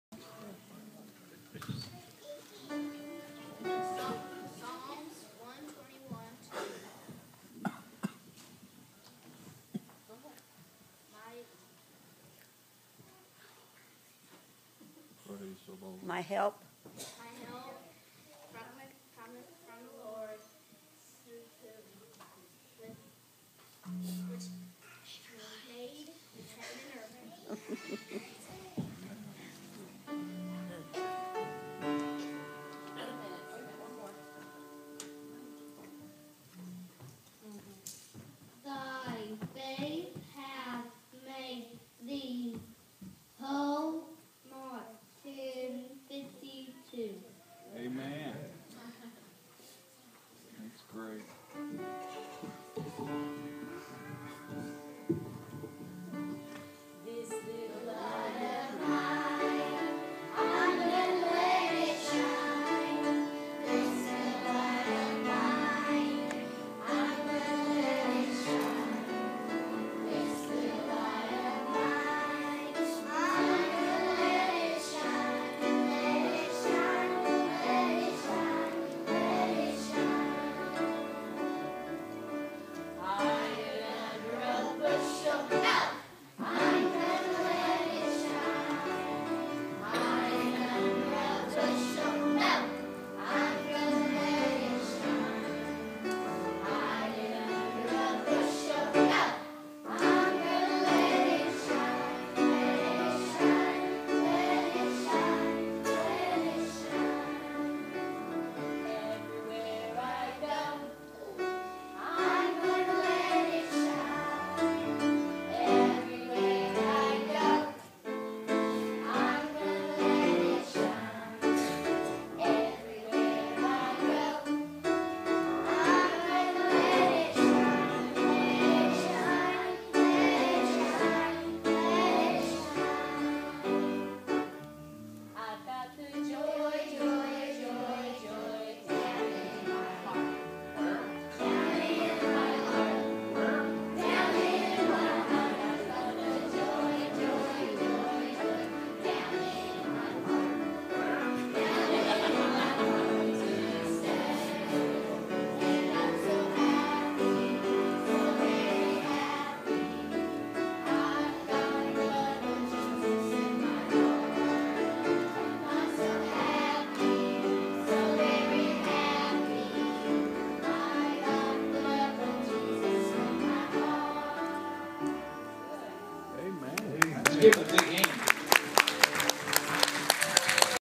ECOG Eldorado Church of God - Kids
Kids from our Sunday School classes singing in front of the church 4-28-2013.